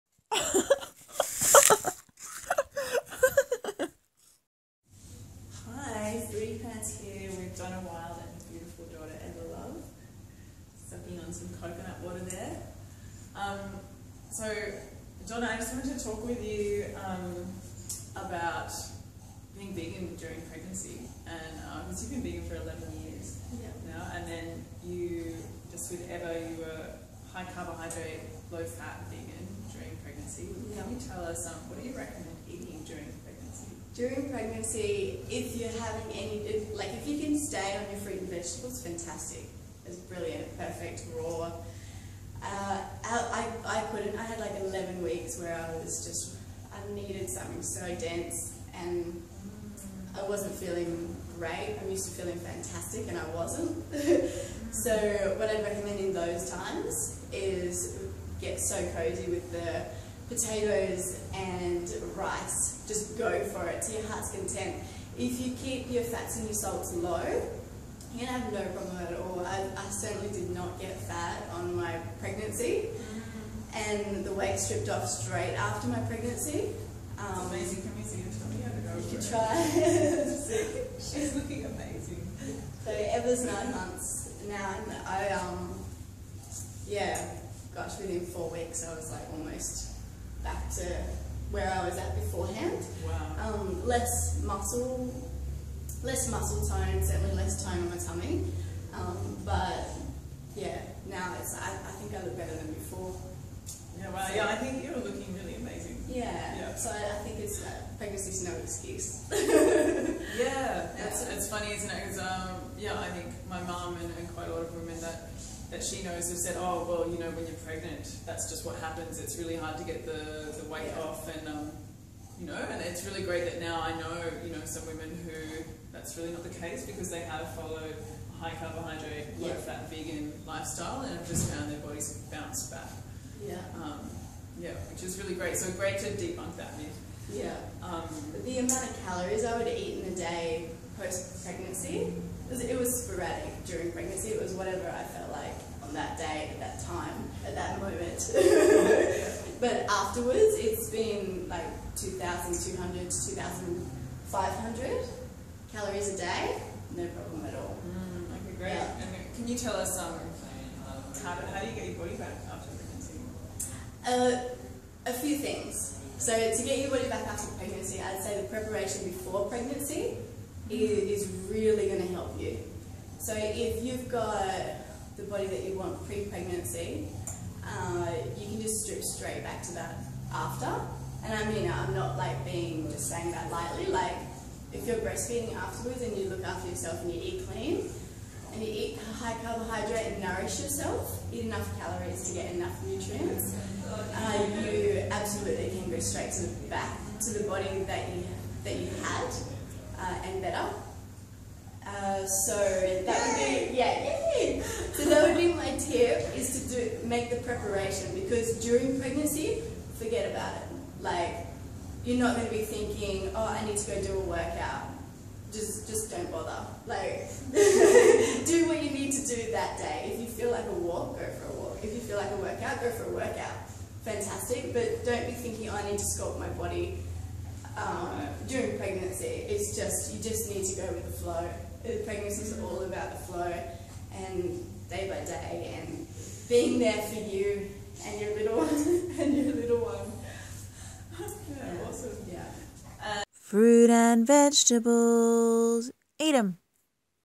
How To Get You Body Back After Childbirth
The incredibly sexy and vivacious Donna Wild speaks on how to have your body literally bounce back after childbirth.  Yay!!